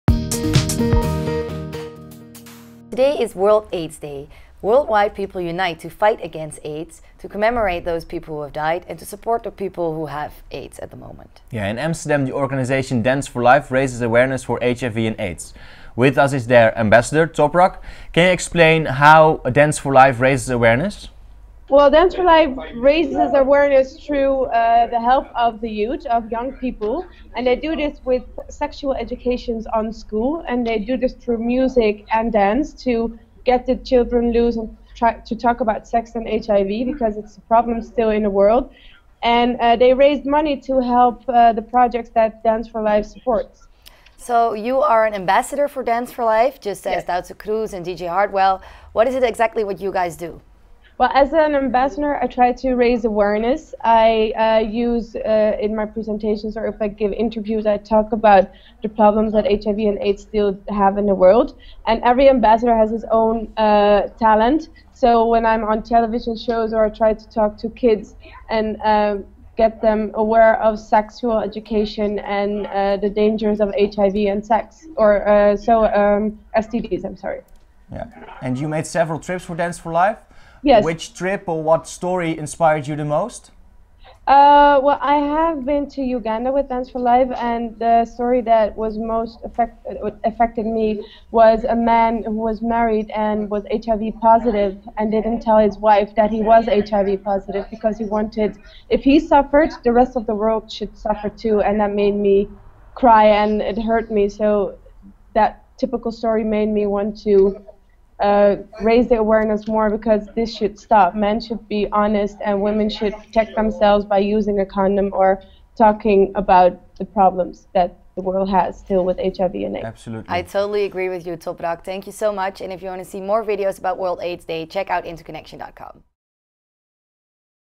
Combating HIV Problems Through Dancing
IntoLife visited the dance4life event in Amsterdam’s HMH. Dance4life is a global organization which is dedicated to spreading awareness about HIV and AIDS. They do this through sex education in schools, using music and dance to involve and inspire young people. Dance4life motivates young people to take action to stop the worldwide spread of HIV and AIDS. Half of the 6.000 people infected with HIV every day are under 25 years old.

The initiative is active in 20 countries, spread over 5 continents. So far they have reached 1.6 million young people and over 600.000 of them took action for a world without AIDS. Through global efforts the number of new HIV infections is 33% lower than it was in 2001. We spoke with dance4life ambassador Toprak Yalçiner about her work for this worthy cause. Toprak explained to us how this organization works and what her role is as an ambassador.